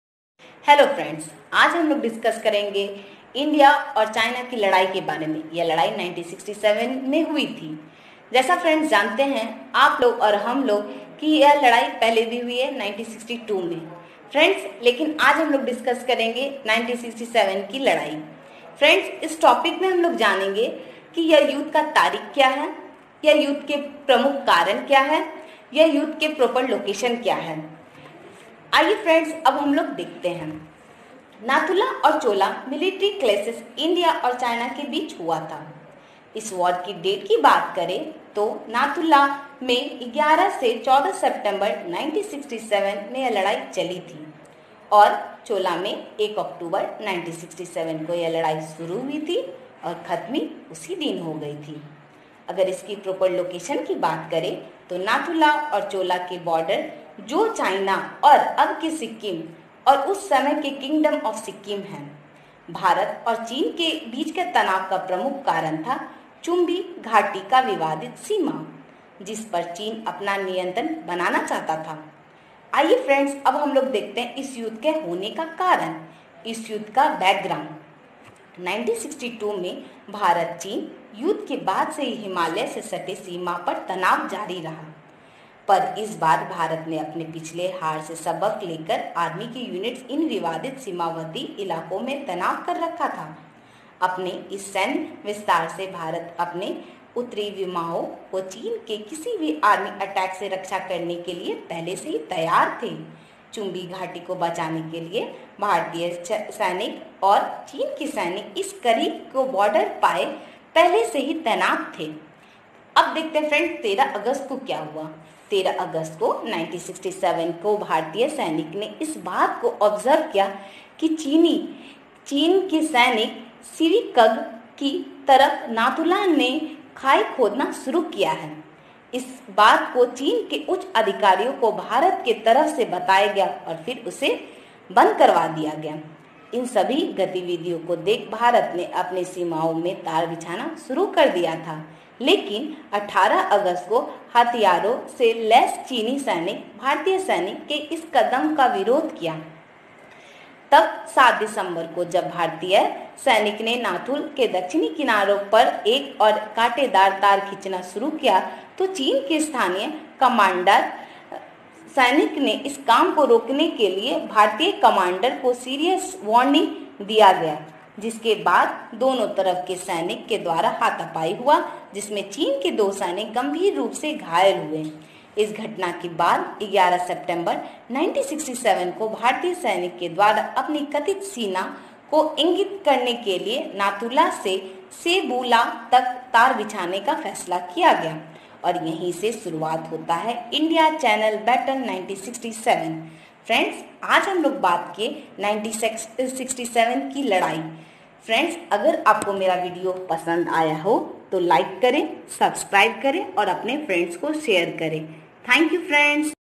||1967 India-China war|| युद्ध के पहले की कहानी
1962 Sino_Indian War, tensions continued to run high along the Himalayan border shared by India and China. Influenced by its previous defeat, the Indian Army raised a number of new units, nearly doubling their deployed forces along the disputed region. As a part of this military expansion, seven mountain divisions were raised to defend India's northern borders against any Chinese attack. Most of these divisions were not based near the border, save for the Chumbi_Valley, where both Indian and Chinese troops are stationed on both sides at close range. Particularly at the Nathu La pass in the valley, alongside the Sikkim-Tibet border, the deployed Chinese and Indian forces are stationed about 20–30 meters apart, which is the closest of anywhere on the 4000 km Sino-Indian border. The border here is said to have remained un-indicated. Chinese held the northern shoulder of the pass, while Indian Army held the southern shoulder. Two major parts of the pass, south and north of Nathu La, namely Sebu La and Camel’s back, were held by the Indians. From 1963, small-scale clashes in the region were frequently reported in the press. On 16 September 1965, during the Indo-Pakistani War of 1965, China issued an ultimatum to India to vacate the Nathu La pass. However, GOC 17 mountain division's Major General, Sagat Singh, refused to do so, arguing that Nathu La was on the watershed which comprised the natural boundary.

Starting from 13 August 1967, Chinese troops started digging trenches in Nathu La on the Sikkimese side. Indian troops observed that some of the trenches were "clearly" to the Sikkemese side of the border, and pointed it out to the local Chinese commander, who was asked to withdraw from there. Yet, in one instance, the Chinese filled the trenches again and left after adding 8 more loudspeakers to the existing 21. Indian troops decided to stretch a barbed wire ElAdbuda along the ridges of Nathu_La in order to indicate the boundary.

                      My last video links